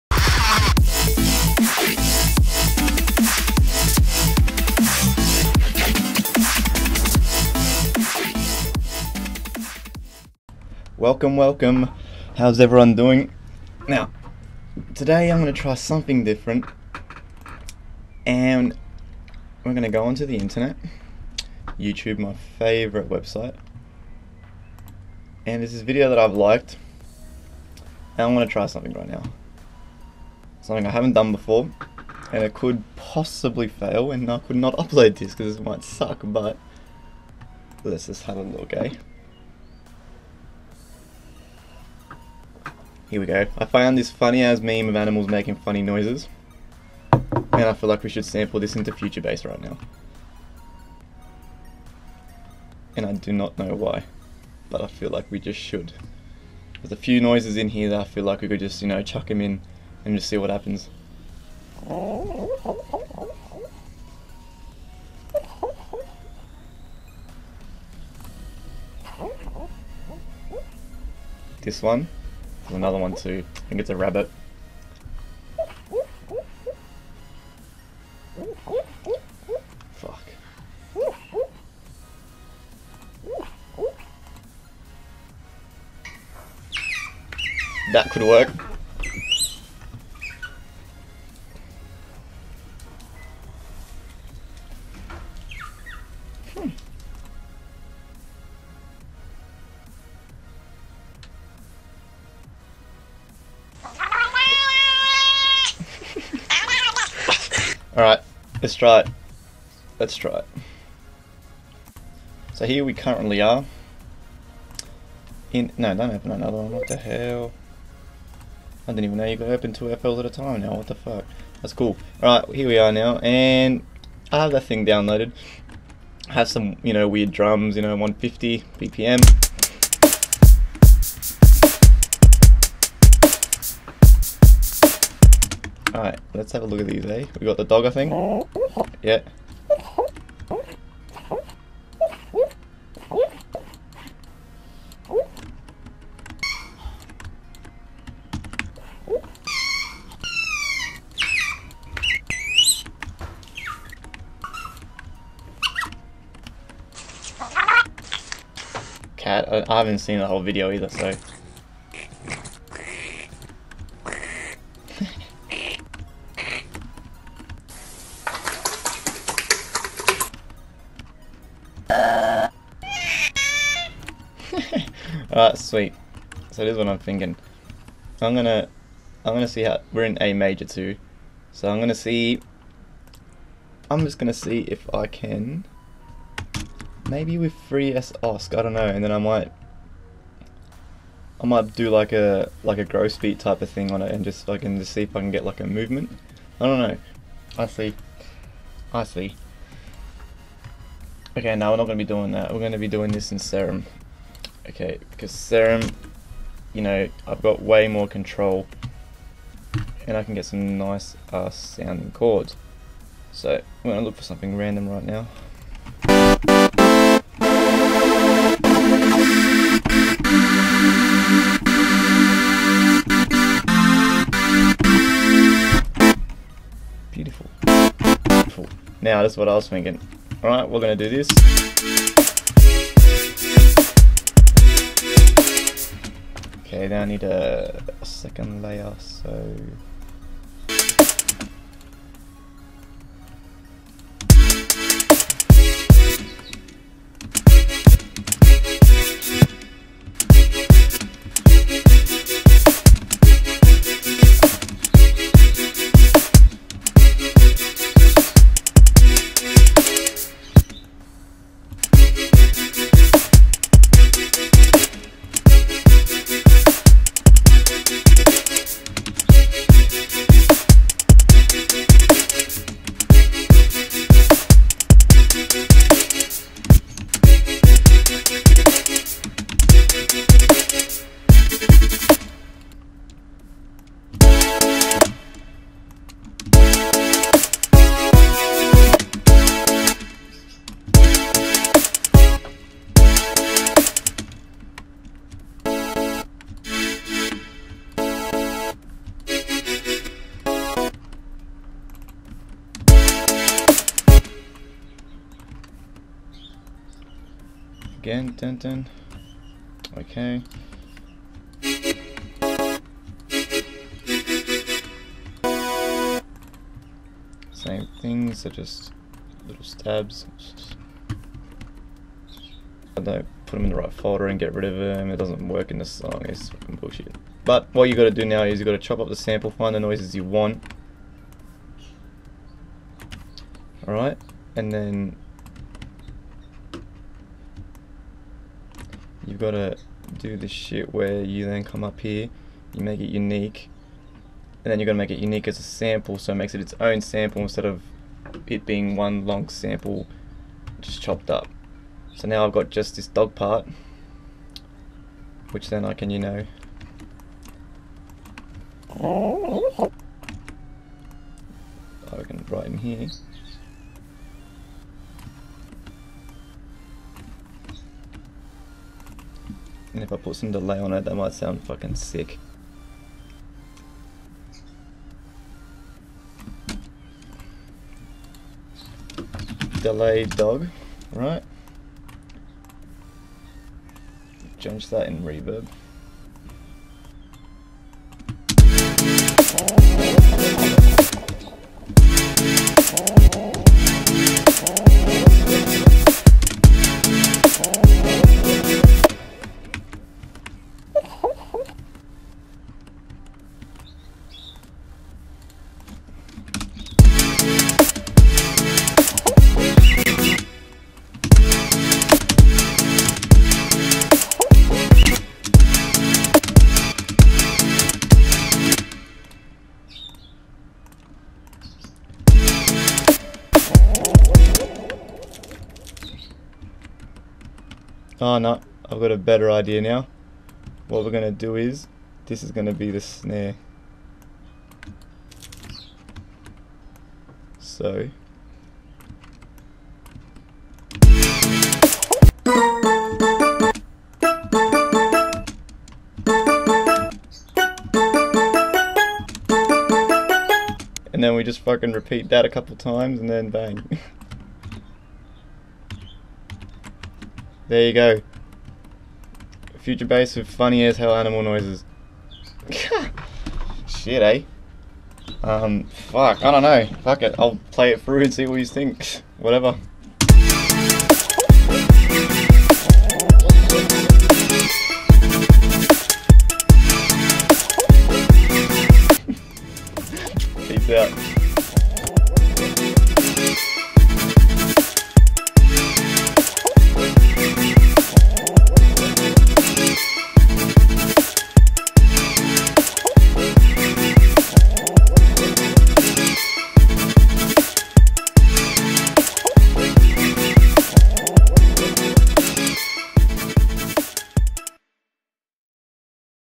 Future Bass With Funny Animal Sounds
```````````````````READ DESC``````````````````````````

So i tried something new. Funny how watching an animal meme can give me an idea for a video hahahaha!
Hope you enjoyed and had a laugh ! What could we sample next ?